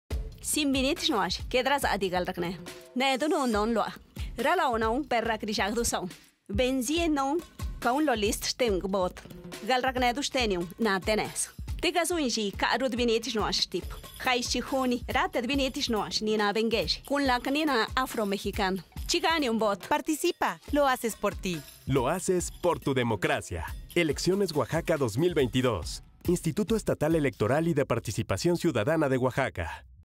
Promoción del Voto Mujeres Indígenas y Afromexicanas - Zapoteco
El domingo 5deJunio todas las mujeres tienen derecho a votar de manera libre, secreta y directa. Todas las personas debemos vigilar que las oaxaqueñas ejerzan su derecho al voto sin ningún impedimento. Sin mujeres no hay democracia. ¡Vota, hazlo PorTuDemocracia!